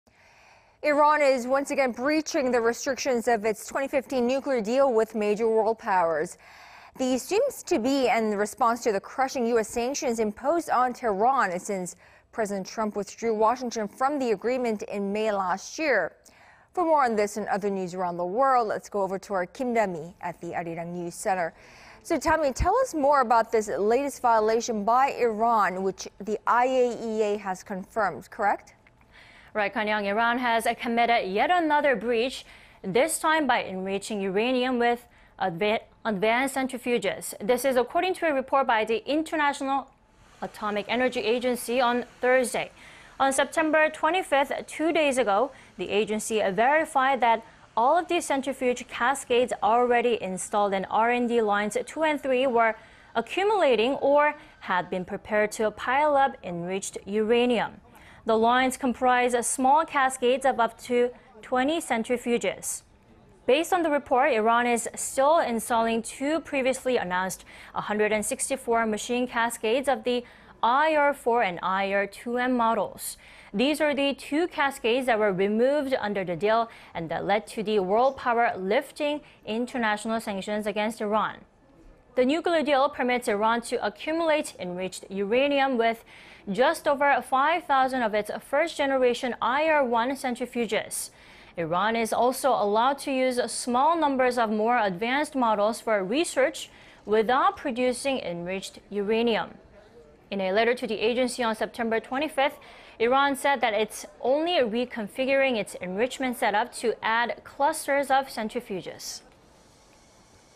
Iran commits new breach of fraying nuclear deal, expands uranium enrichment: IAEA
IAEA "이란, 고성능 원심분리기 사용…핵합의 위반"
  Iran is once again breaching the restrictions of its 2015 nuclear deal with major world powers.
This seems to be in response to the crushing U.S. sanctions imposed on Tehran since President Trump withdrew Washington from the agreement in May last year.
For more on this and other news around the world, let's turn to our Kim Da-mi.
Da-mi, tell us more about this latest violation?

  Connyoung, Iran has committed yet another breach...this time by enriching uranium with advanced centrifuges.
  This is according to a report by the International Atomic Energy Agency on Thursday.
On September 25th, two days ago, the agency verified that all of the centrifuge cascades already installed in R&D lines 2 and 3 were accumulating,... OR had been prepared to pile up enriched uranium.
The lines comprise small cascades of up to 20 centrifuges.
  Based on the report, Iran is still installing two previous announced 164-machine cascades of the IR-4 and IR-2m models.
These are the two cascades that were removed under the deal and that lead to the world power lifting international sanctions against Iran.
The nuclear deal permits Iran to accumulate enriched uranium with just over 5-thousand of its first-generation IR-1 centrifuges.
Iran is also allowed to use small numbers of more advanced models for research...WITHOUT producing enriched uranium.
In a letter to the agency on September 25th, Iran said that it's only reconfiguring its enrichment set-up to add clusters of centrifuges.